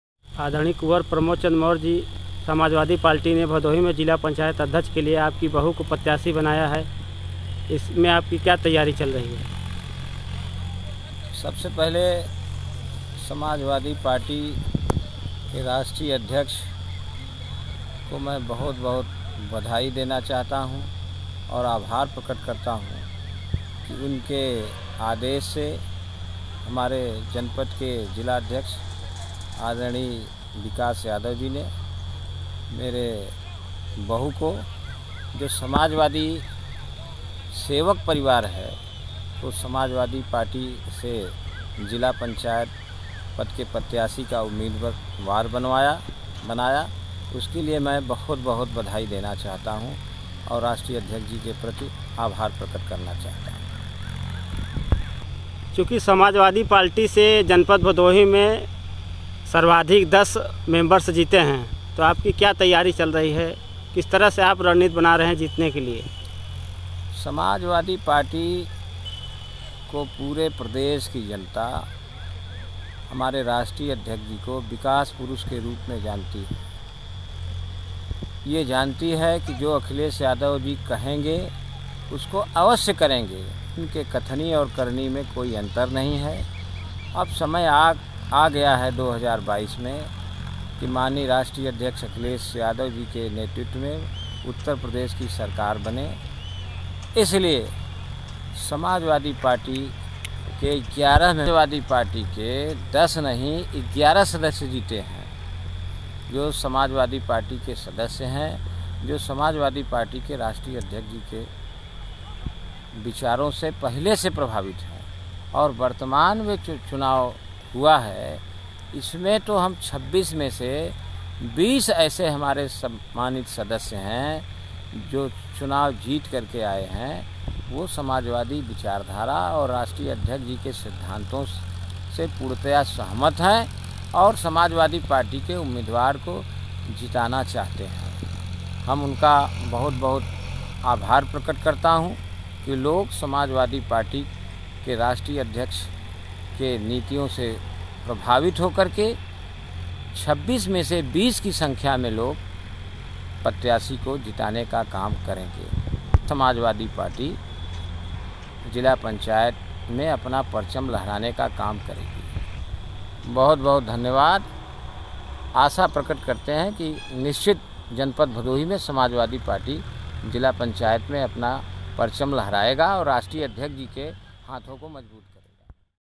Kunwar Pramod Chand Maurya Old Interview in Gyanpur Bhadohi Uttar Pradesh
मिशन 2022 कुंवर प्रमोद चंद्र मौर्य  मिशन 393 ज्ञानपुर विधानसभा चुनाव 2022 भदोही उत्तर प्रदेश